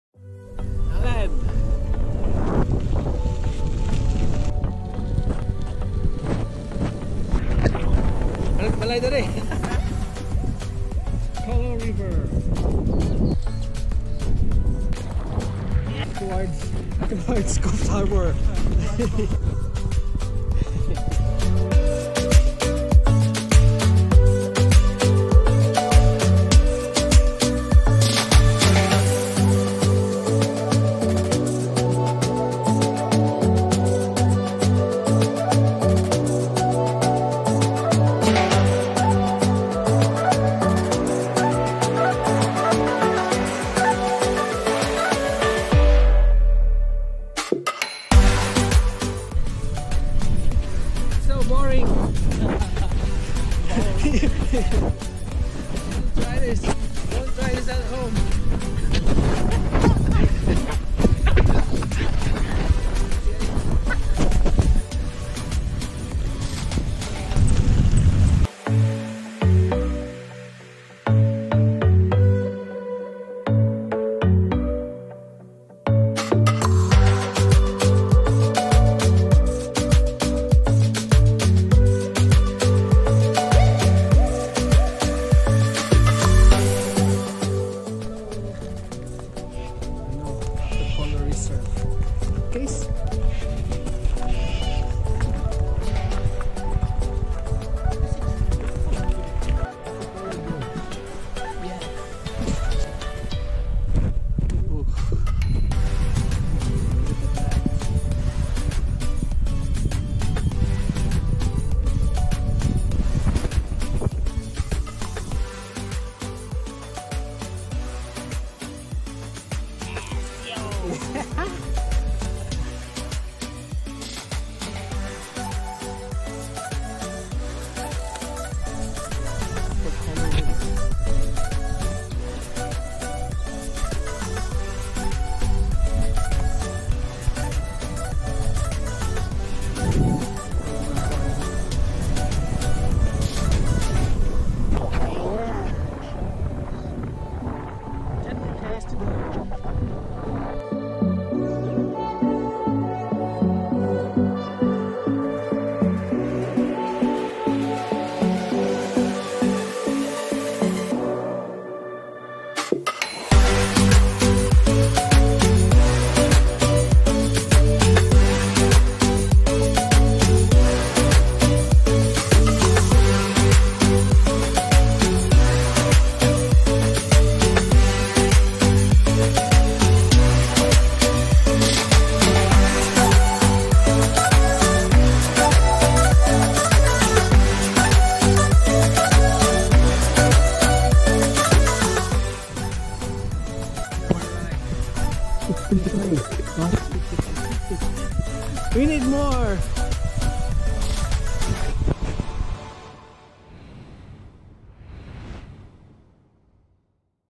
Colo River gravel and MTB ride
Gravel and MTB ride along the Colo River, NSW. 20 July 2020.